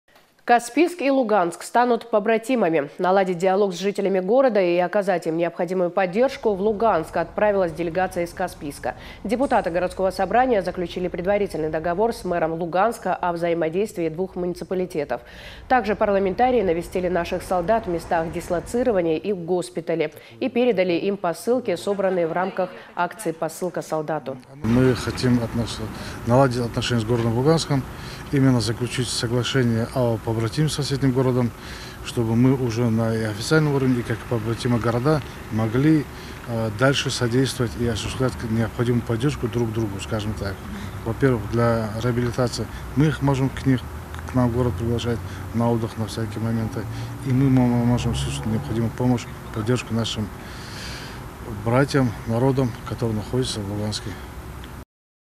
Каспийск и Луганск станут побратимами
Наладить диалог с жителями города и оказать им необходимую поддержку в Луганск отправилась делегация из Каспийска. Депутаты городского собрания заключили предварительный договор с мэром Луганска о взаимодействии двух муниципалитетов.